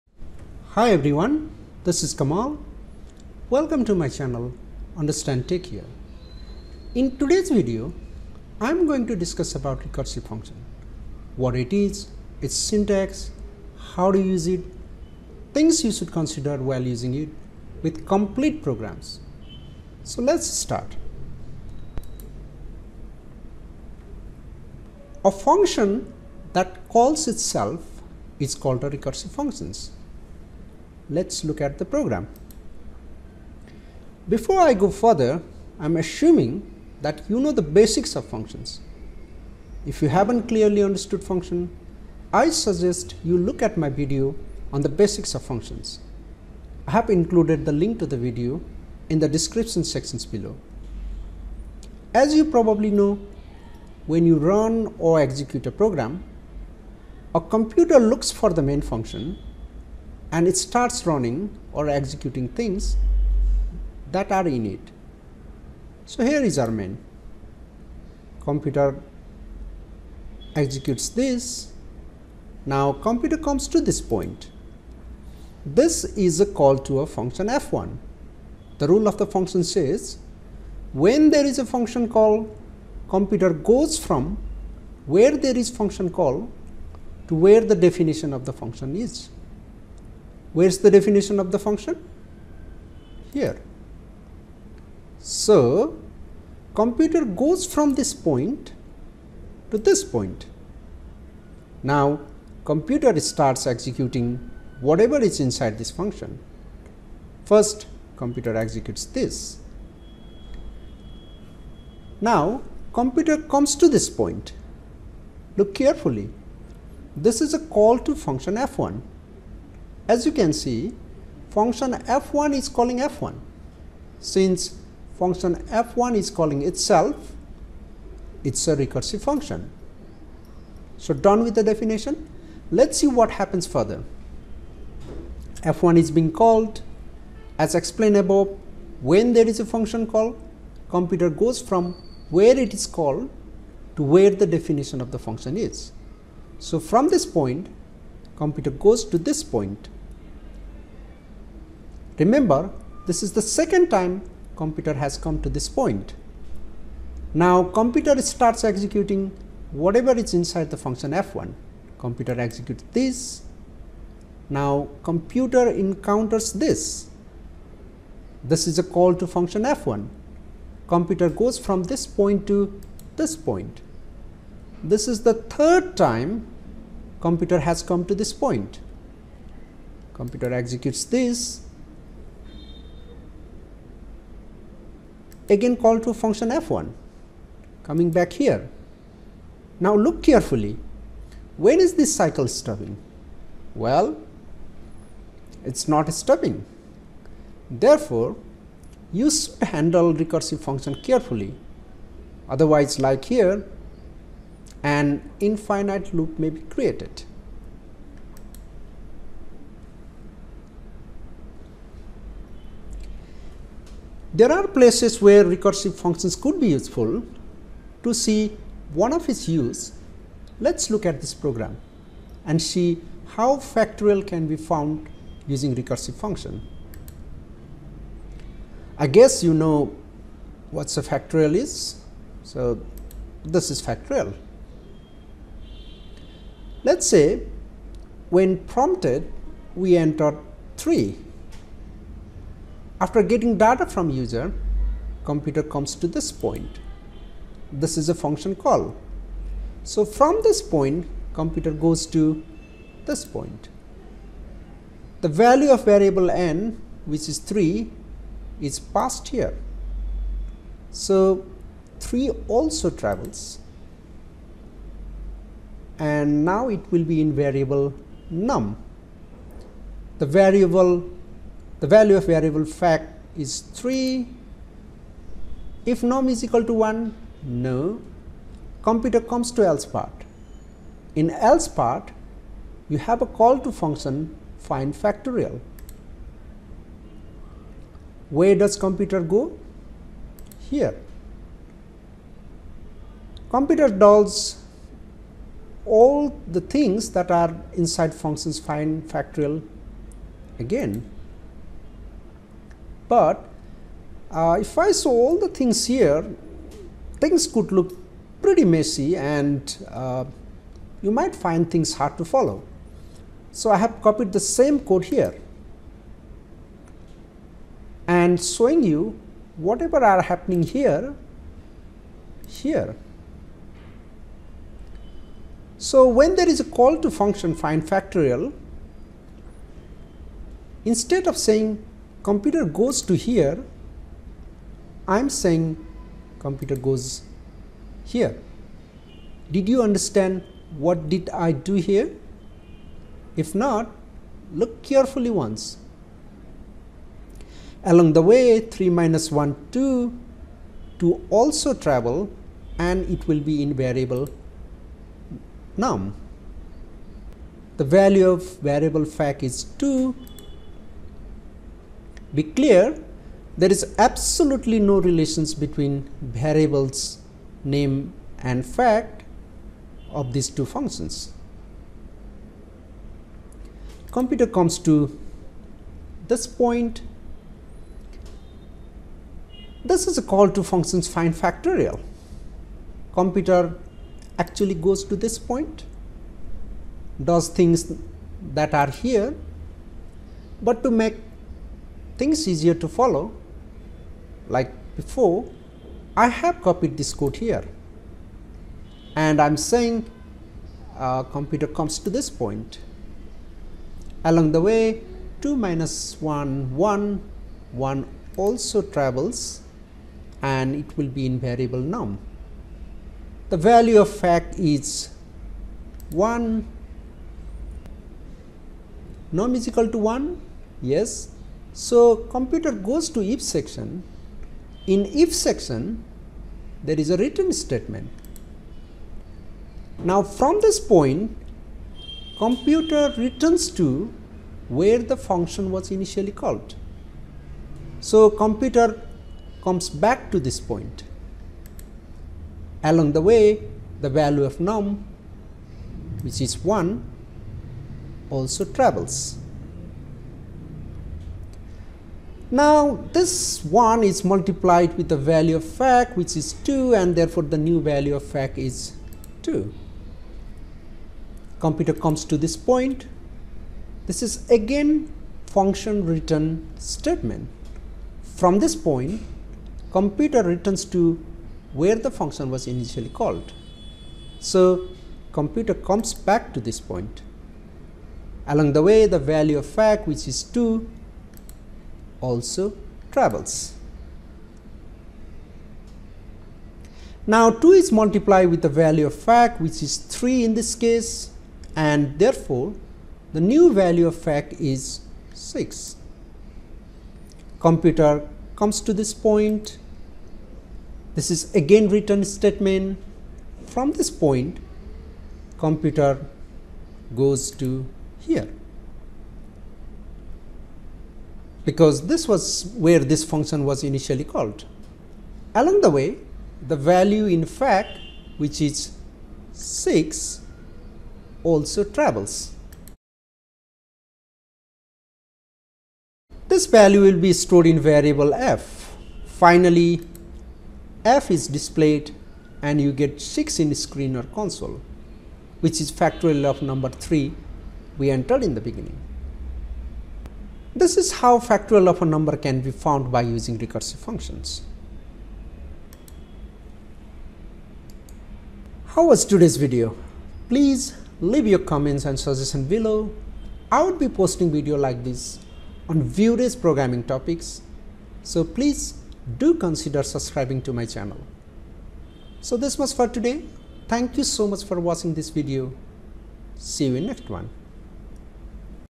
Recursive Function in C: A Step-by-step Animated Explanation
A recursive function is a special type of function that calls itself. This video will explain what is a recursive function, how to use it to solve practical problems like finding factorial, how not to use it to create an infinite loop – through C programs.

If you don't have a clear understanding of function, go through this video first: Function in C

0:00 Introduction
0:24 Definition of recursive function
3:32 Finding factorial using a recursive function

Click on this link (your support help us to keeps us free)